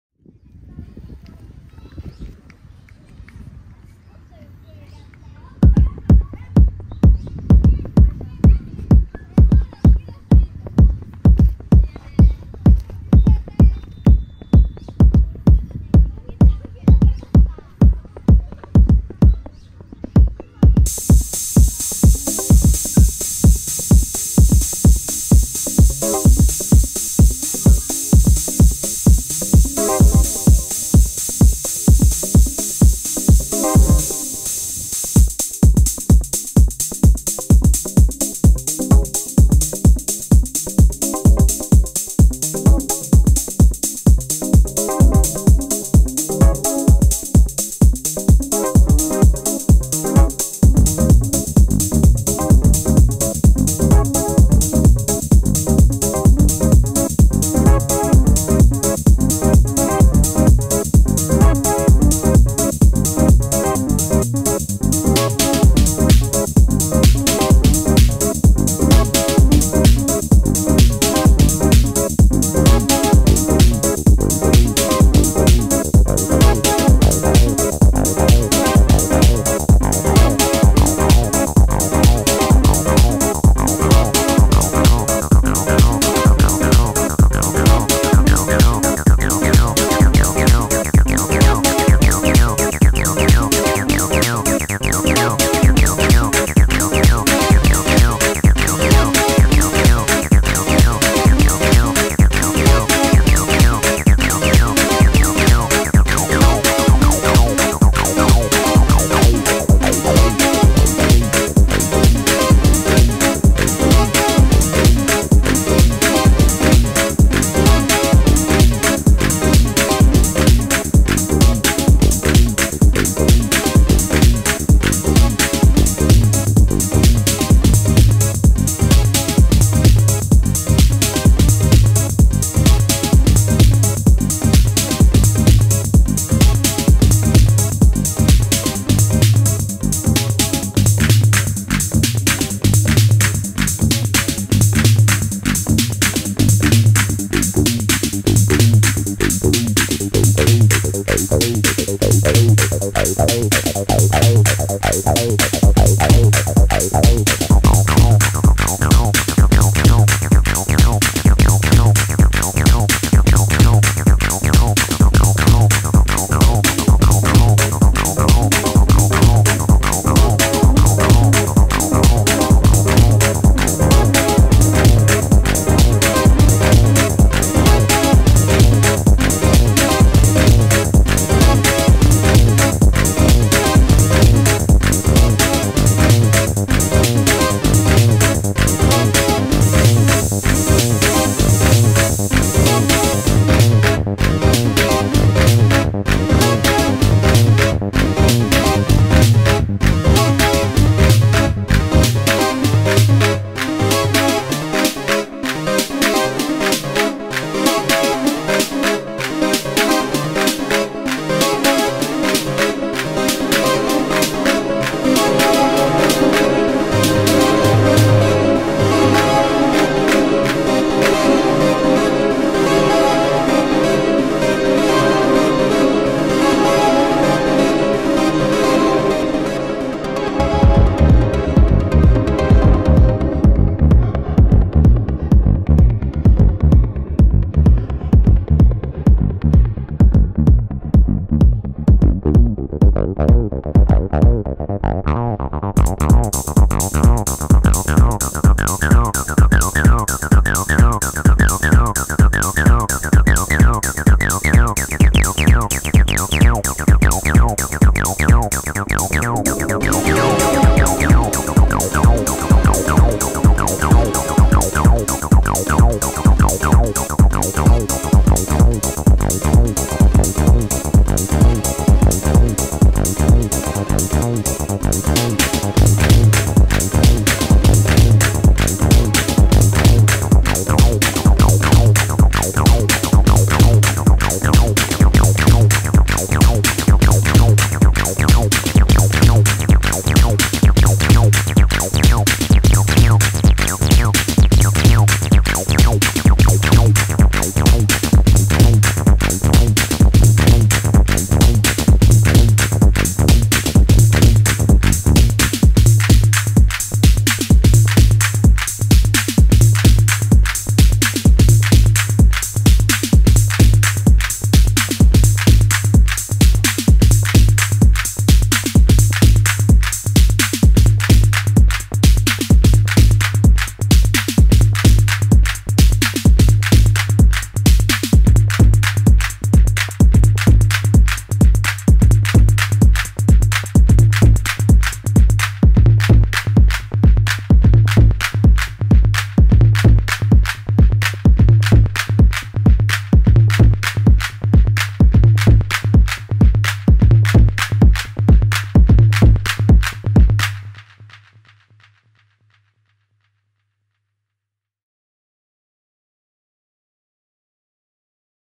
Techno Jam 006 : Rorand Aira Compact T-8 & J-6
2023年7月2日。天気がいいので公園で演奏してみました。横からの撮影で奥のT-8が見えないですがご了承ください。
Roland Aira Compact
Aira T-8